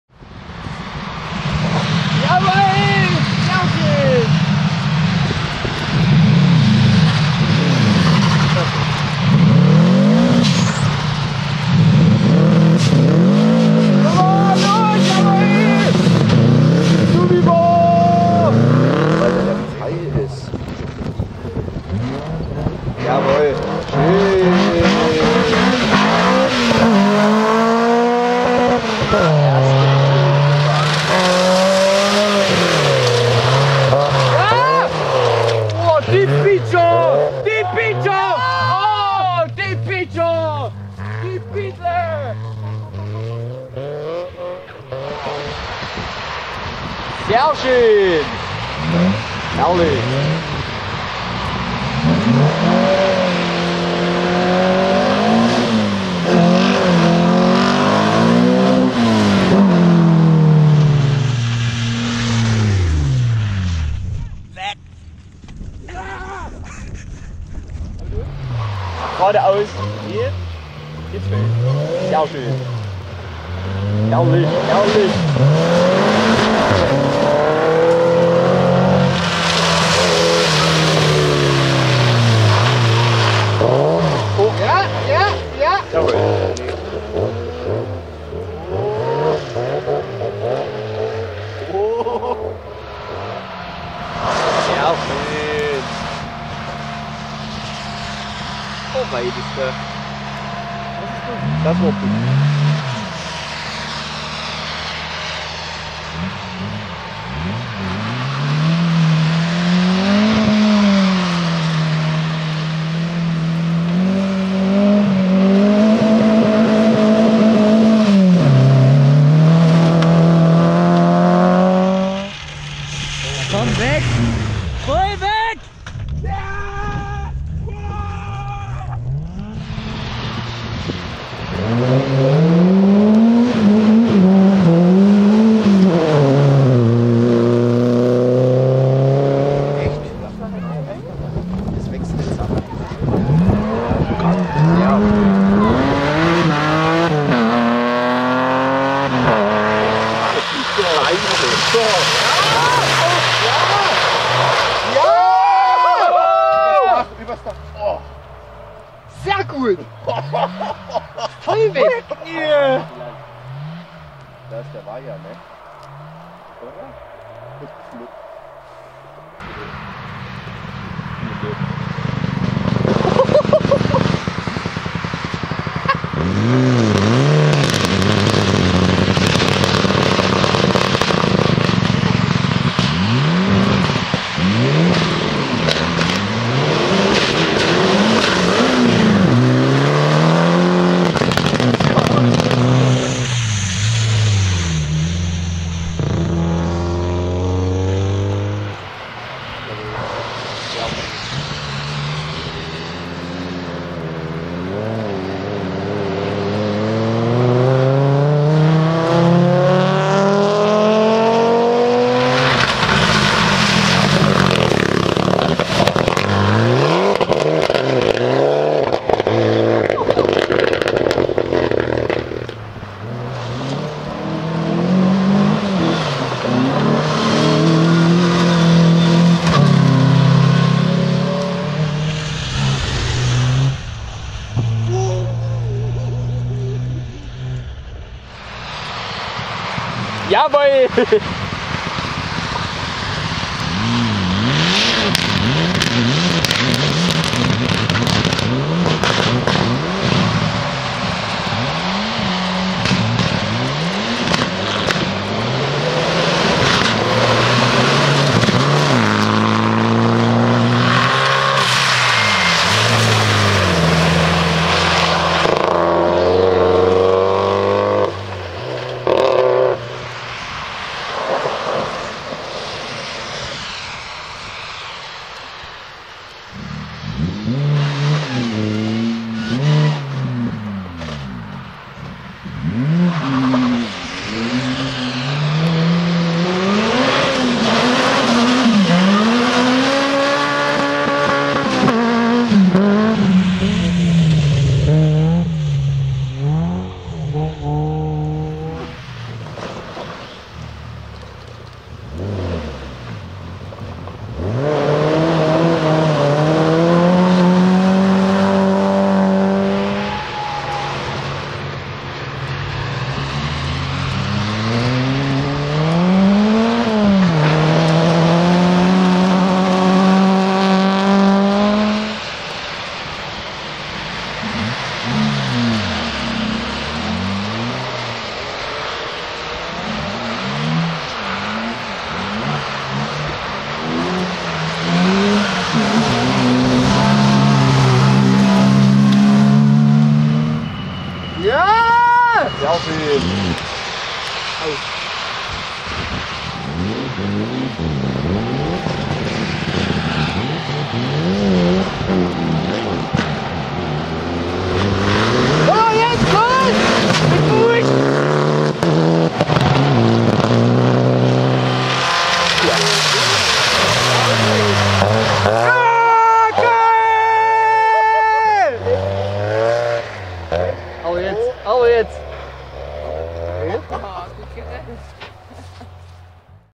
NAVC Fürst Carl Rallye 2022 | CRASHS & MISTAKES
NAVC Fürst Carl Rallye 2022 including 2 crashs and a few offs